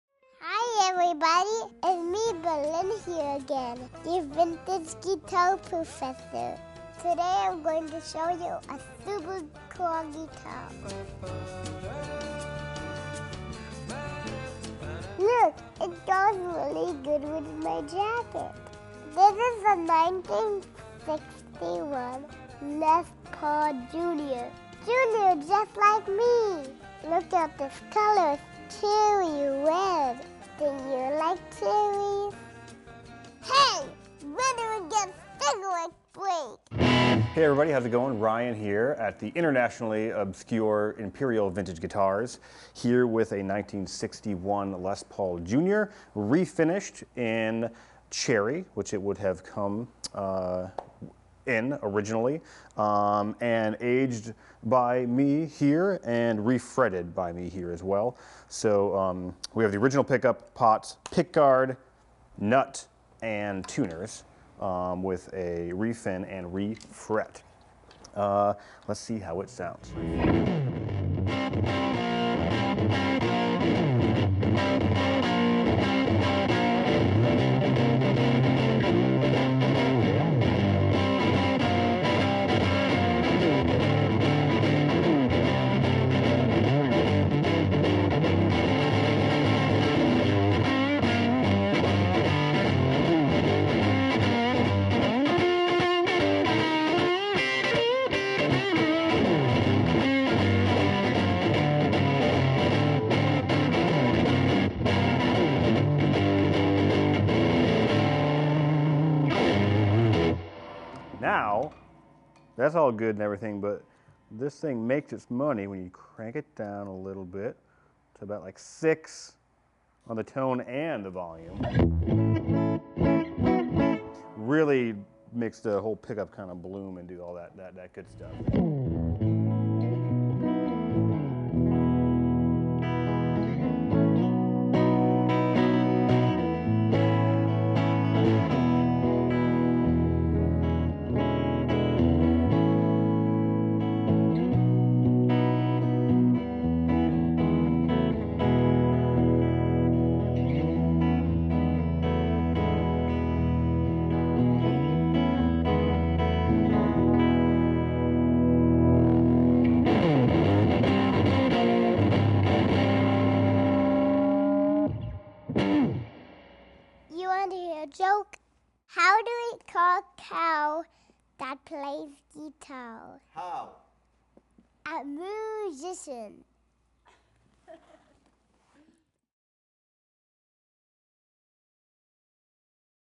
1961 Les Paul Junior
1961 Gibson Les Paul Junior

In Good Overall Condition
Cherry Red Refinish Is In Very Good Condition With Checking, Marks, And Dings (Please See Photos)

Brazilian Rosewood Fingerboard

Original Gibson P-90 Pickup

Original Tailpiece Kluson Tuning Machines

Had A Headstock Repair At One Point (Please See Photos)

Professional Refret

A Case Or Gig Bag Can Be Included For Shipping